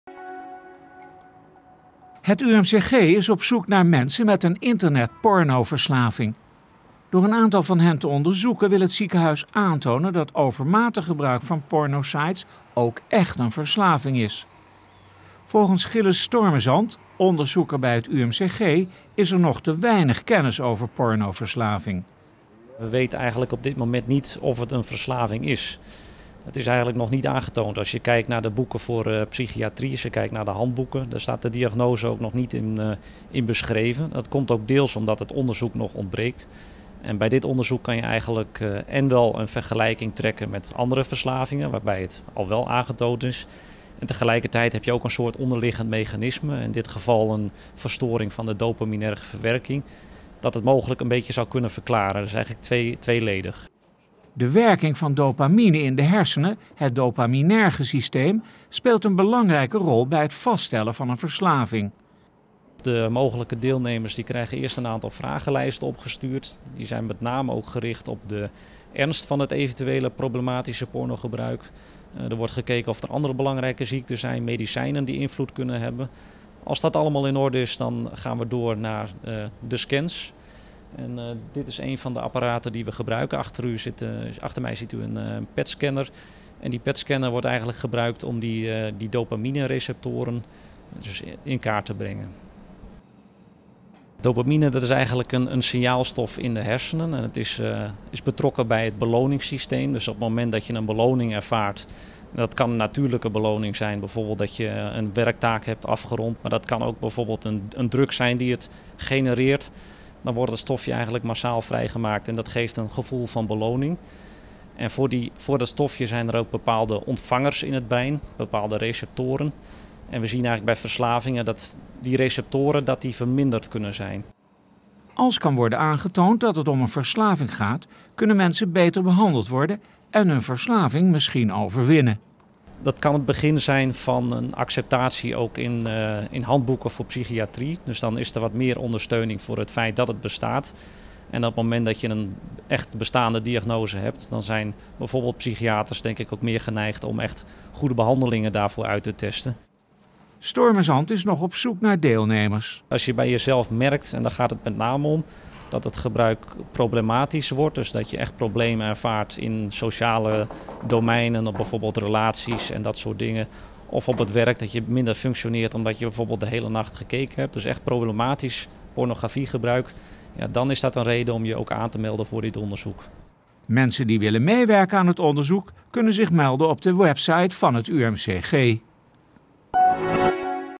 UMCG zoekt pornoverslaafden
Het UMCG is op zoek naar mensen met een internetporno-verslaving.
Door een aantal van deze mensen te onderzoeken wil het ziekenhuis aantonen dat overmatig gebruik van pornosites ook echt een verslaving is. Volgens Gilles Stormezand, onderzoeker bij het UMCG, is er nog te weinig kennis over pornoverslaving.
De werking van dopamine in de hersenen, het dopaminerge systeem, speelt een belangrijke rol bij het vaststellen van een verslaving. Als kan worden aangetoond dat het om een verslaving gaat, kunnen mensen beter behandeld worden en hun verslaving misschien overwinnen.
Stormezand is nog op zoek naar deelnemers, mensen die willen meewerken aan het onderzoek kunnen zich melden op de website van het UMCG.